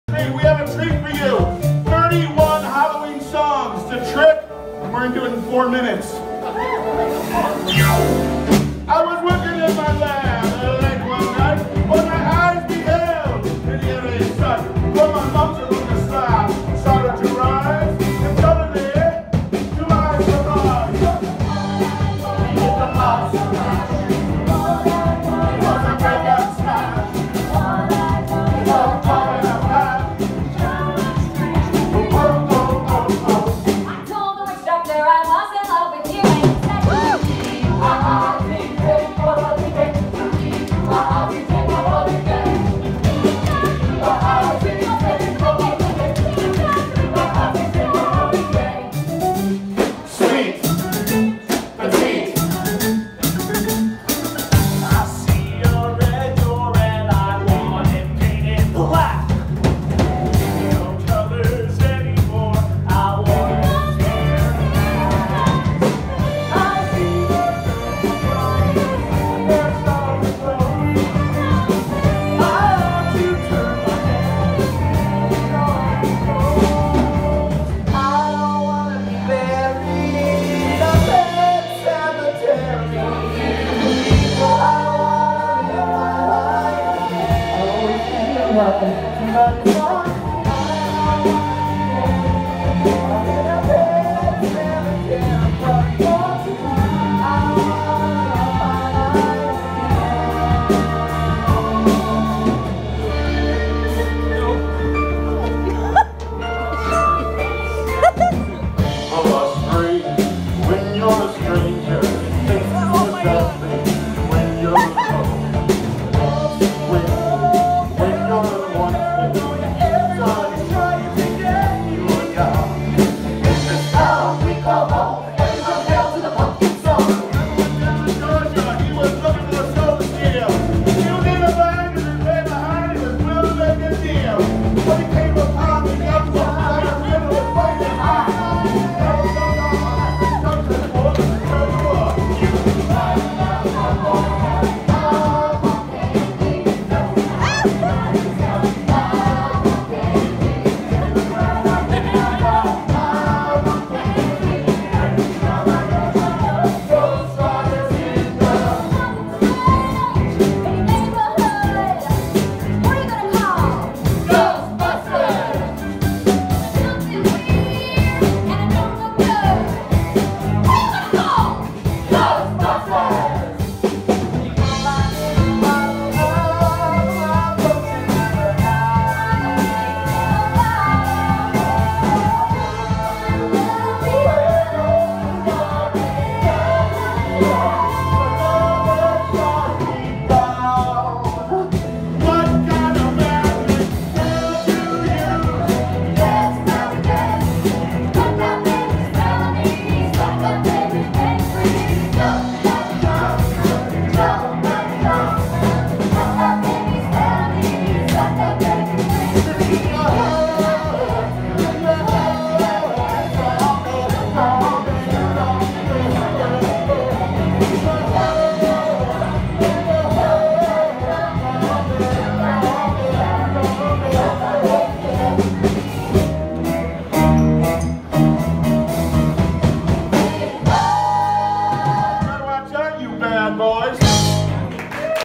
Hocus Pocus Unauthorized Cast - Halloween Mashup (9/21)
The Hocus Pocus Unauthorized Cast (Ashley Argota, Lana McKissack, Tom Detrinis, Jordan Goodsell, Nathan Moore, and Peter Allen Vogt). Hocus Pocus Unauthorized on September 21st 2017 at Rockwell Table & Stage.